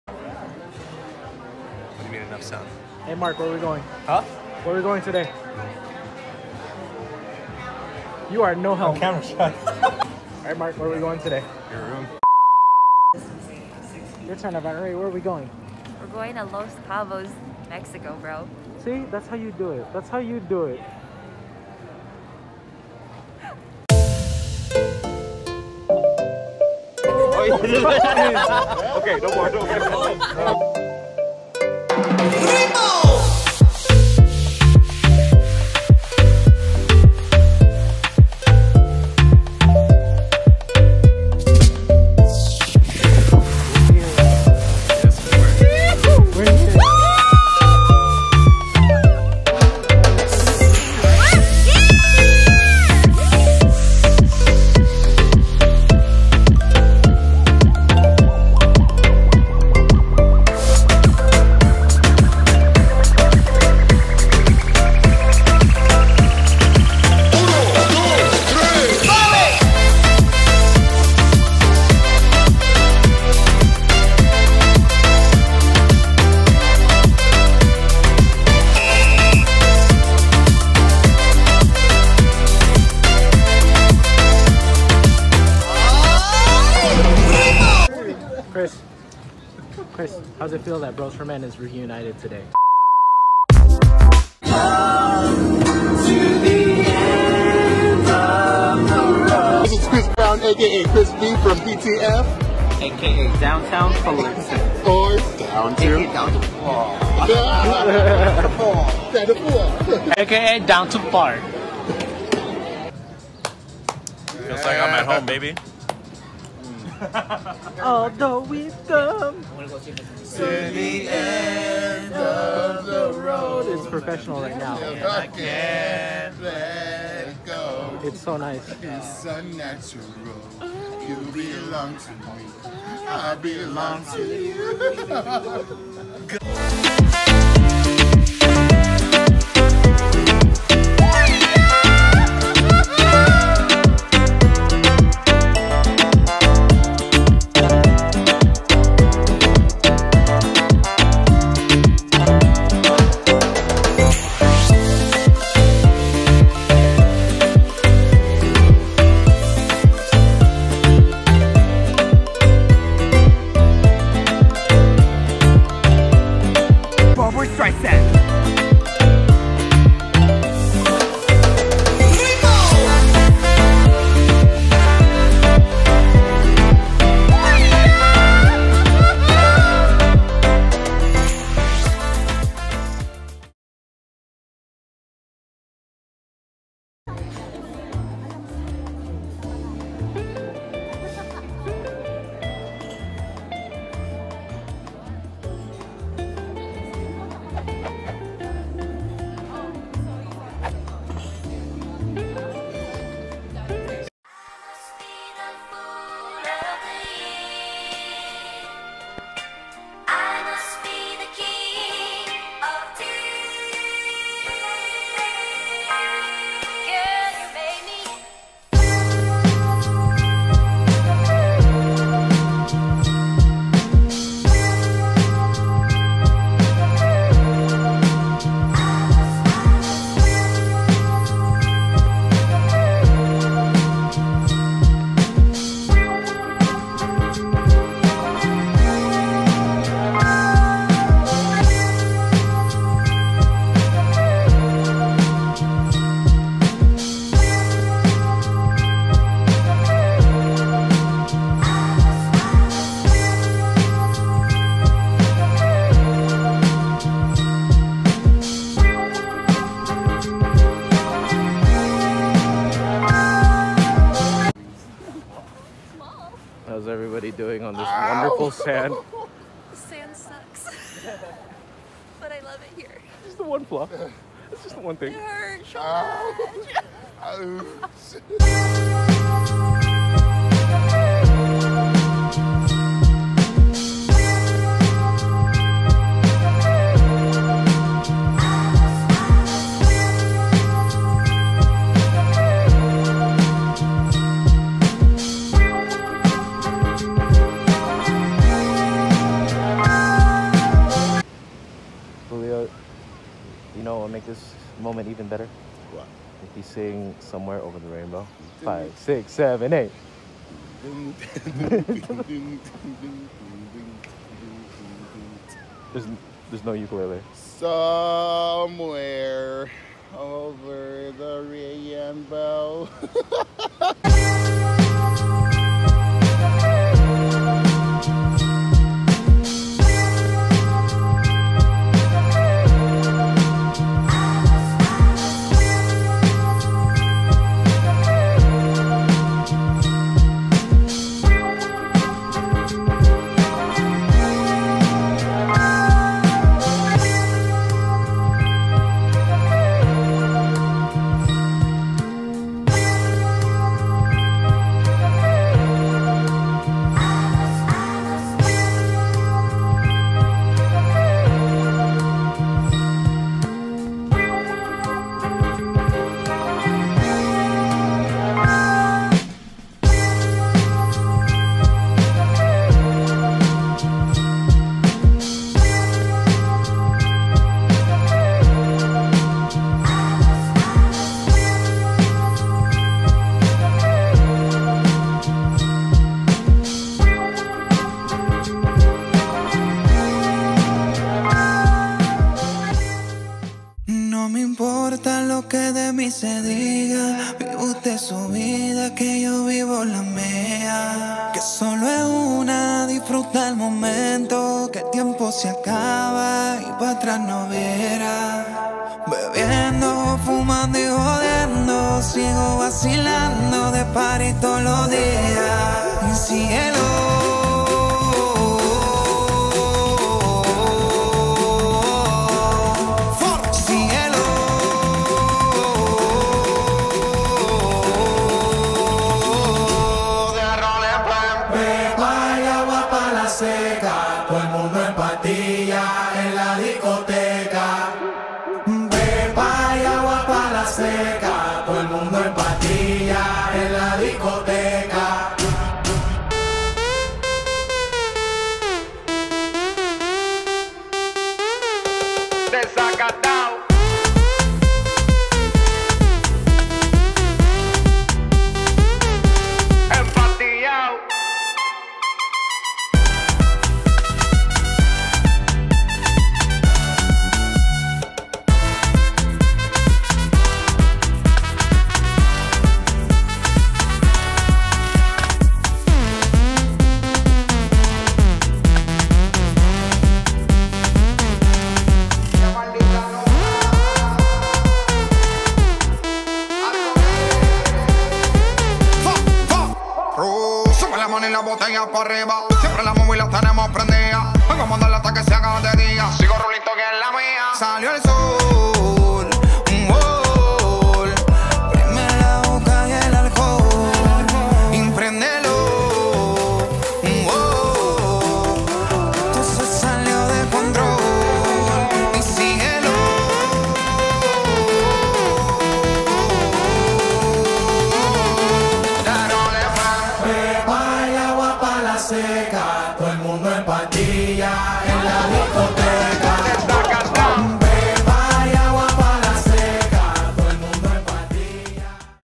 CABO 2022 | LA BODA DE HOMIES
just a vacation vlog of Cabo San Lucas for my homies' wedding